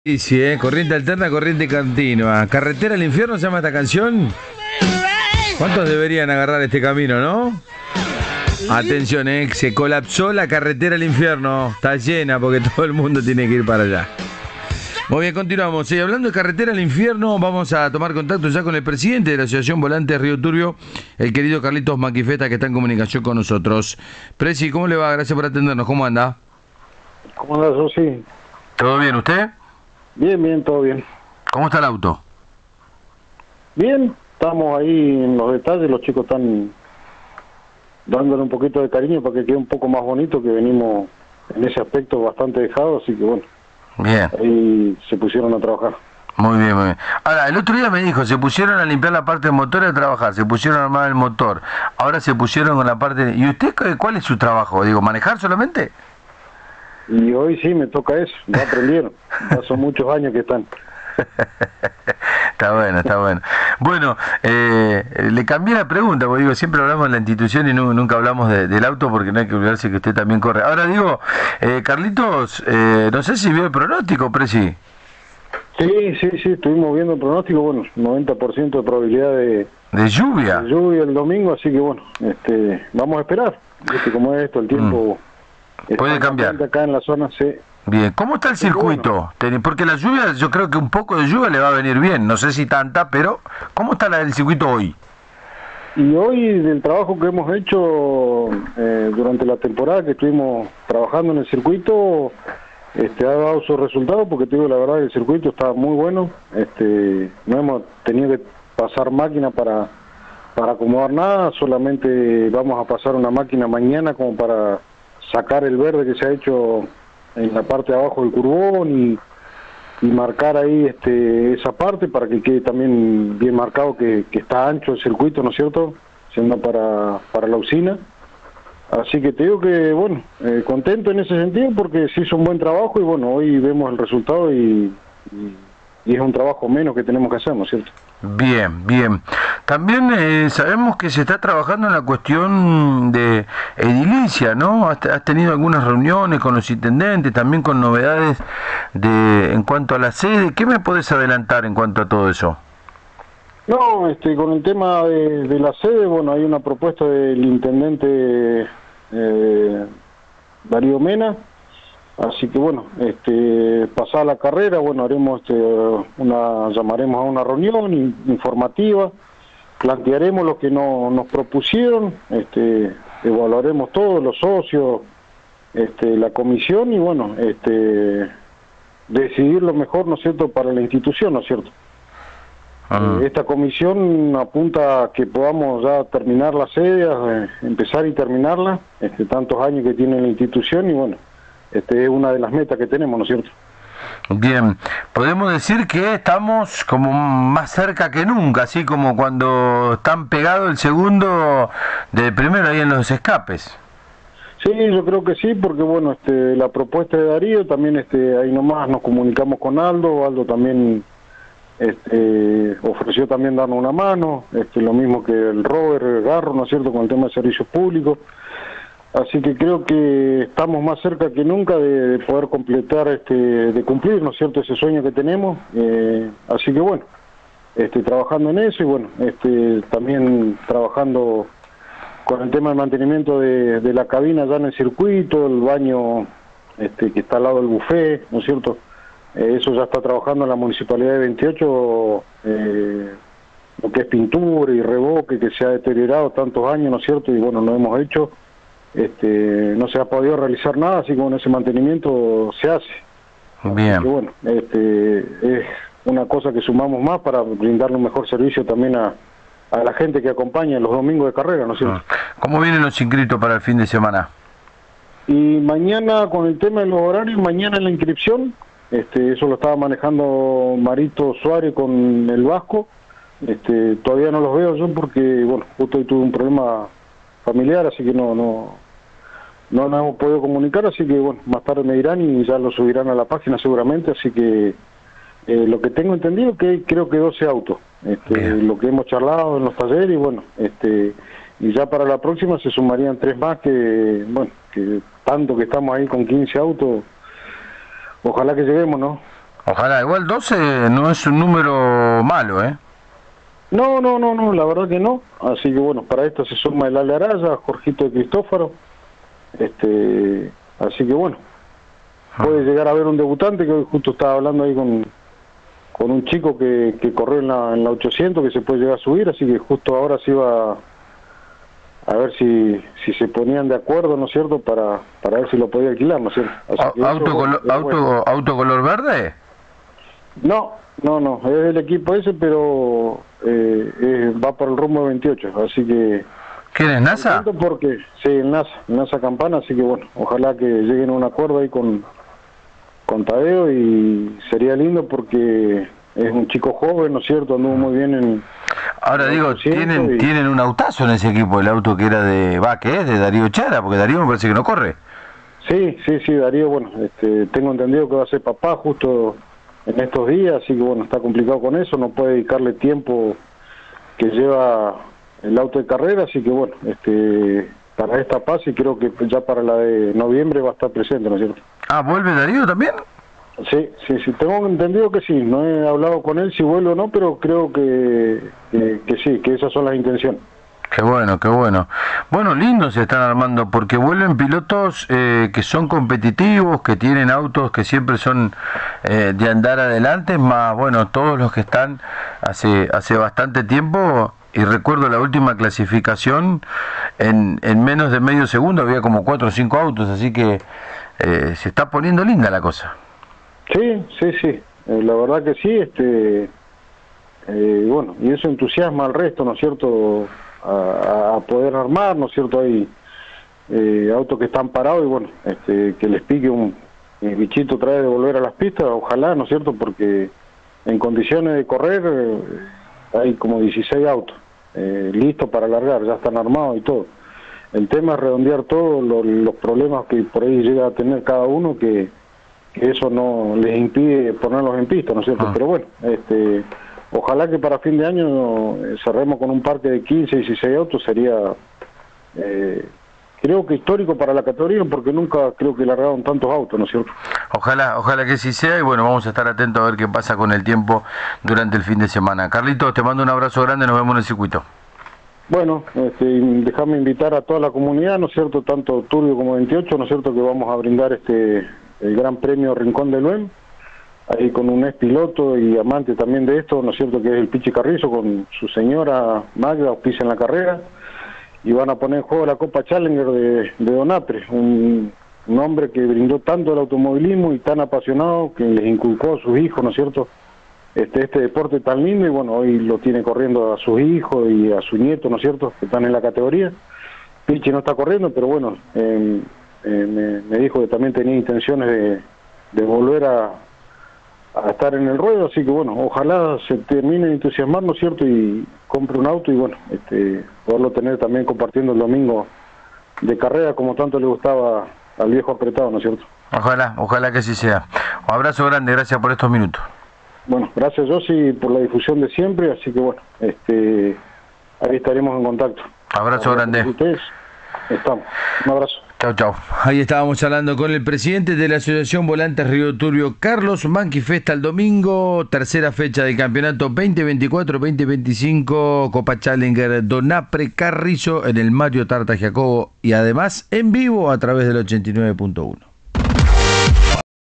EL PORTAL - En comunicación con Carlos Mangifesta Presidente de la Asociación de Volantes de RT
En comunicación con Carlos Mangifesta Presidente de la Asociación de Volantes de Río Turbio.